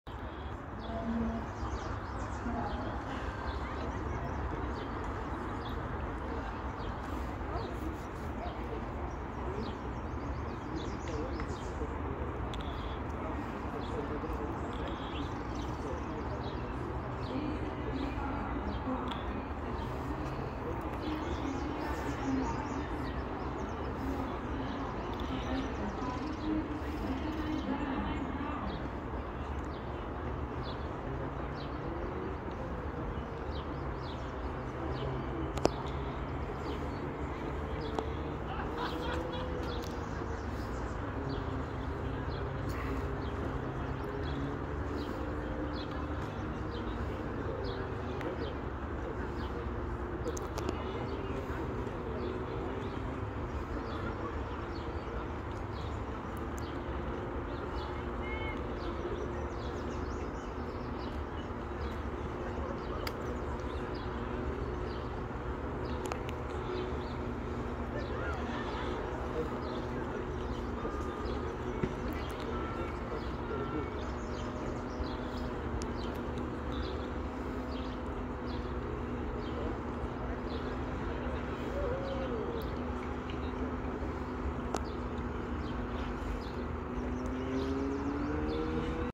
Ben Brown Va'a Canoe 200m Race 22/06/24
I shaved almost 3 seconds off from yesterday. Best technical 200m I have ever done. We need to make more equipment adjustments throughout the summer to get the speed and times desired. Time of 1:17.28 with a headwind.